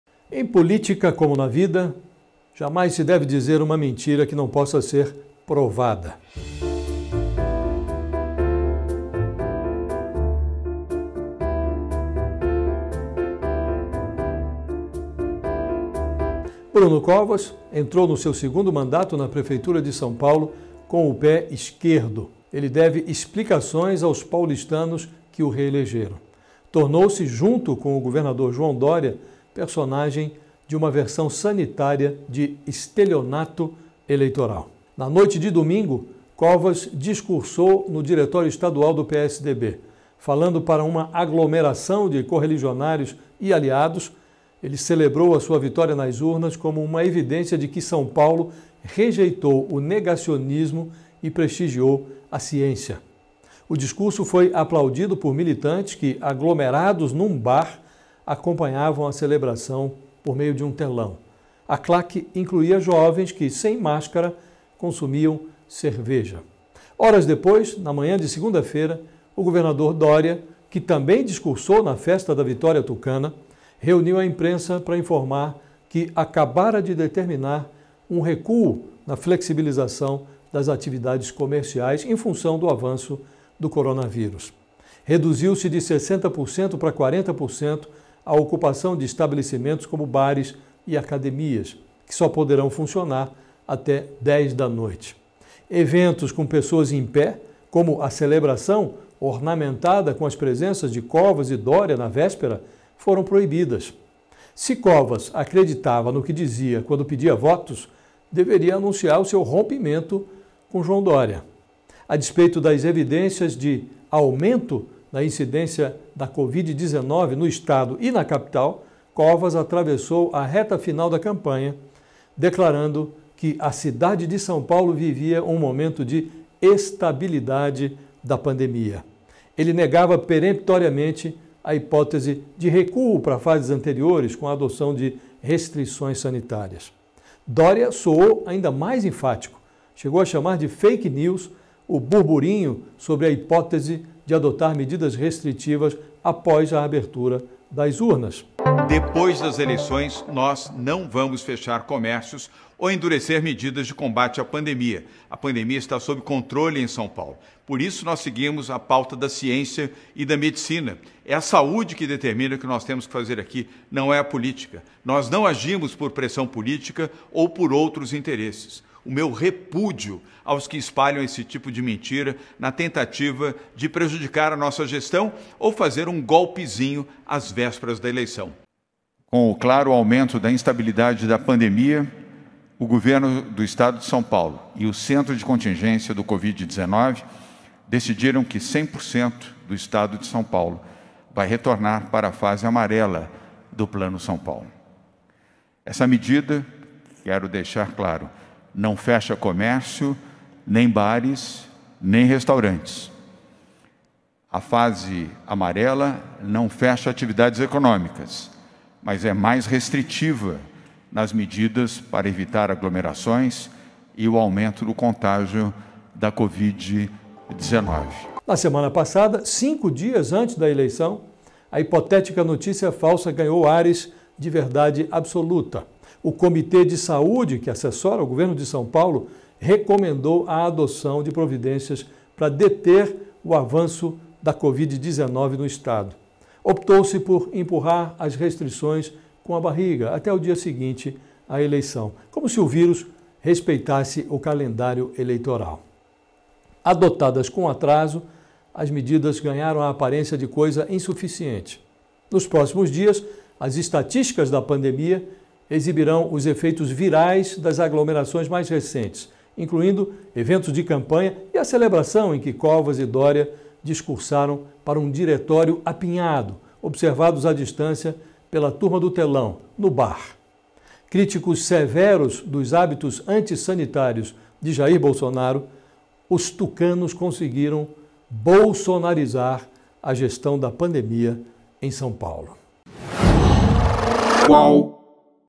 João Doria e Bruno Covas bolsonarizaram a gestão do vírus | Josias de Souza
"Em política, como na vida, jamais se deve dizer uma mentira que não possa ser provada. Bruno Covas entrou no seu segundo mandato na prefeitura de São Paulo com o pé esquerdo. Deve explicações aos paulistanos que o reelegeram. Tornou-se, junto com o governador João Doria, personagem de uma versão sanitária de estelionato eleitoral", analisa Josias de Souza em sua coluna em vídeo desta semana.